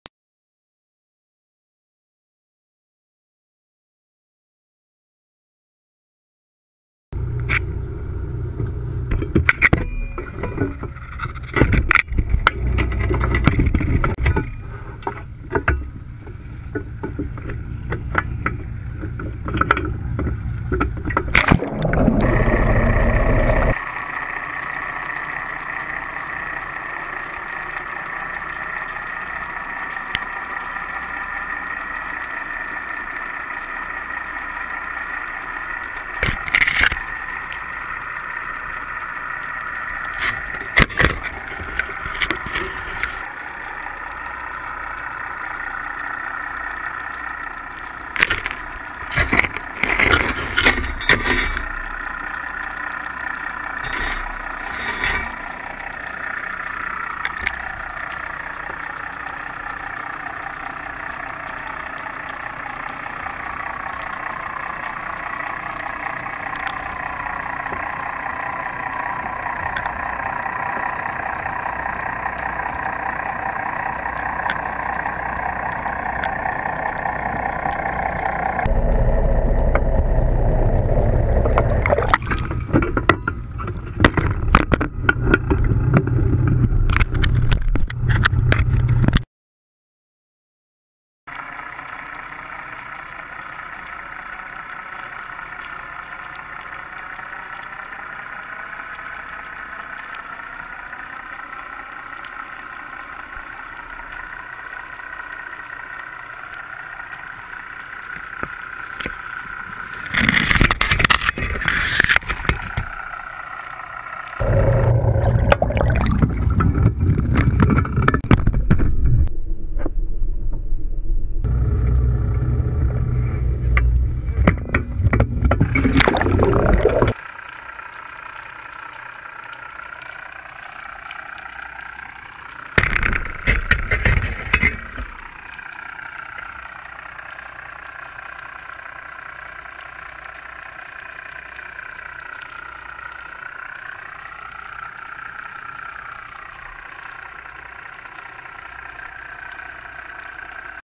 CCFRP GoPro CTD video footage
Lead Cal Poly SLO CCFRP science technician Grant Waltz compiled and edited this GoPro video footage. It was filmed from our CTD device in the Piedras Blancas reference cells. Enjoy!